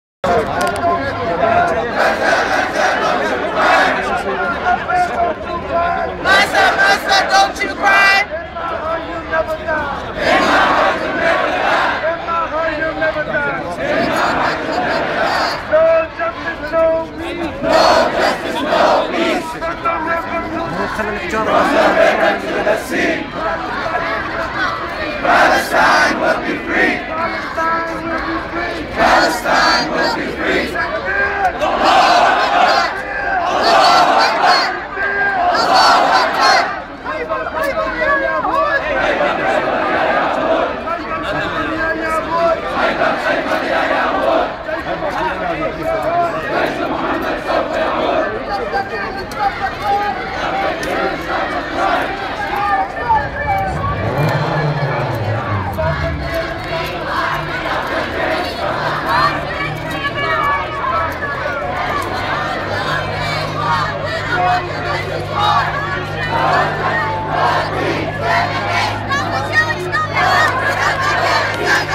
New Orleans Protest for Gaza - Free Palestine Part 2
So far hundreds of civilians have been killed in Gaza. Five sisters in one family, four other children in another home, two children on a cart drawn by a donkey. Universities, colleges, police stations, roads, apartment buildings were all targeted. The UN Special Rapporteur on Human Rights in the Occupied Palestinian areas issued a statement that
"The Israeli airstrikes on the Gaza Strip represent severe and massive violations of international humanitarian law as defined in the Geneva Conventions, both in regard to the obligations of an Occupying Power and in the requirements of the laws of war."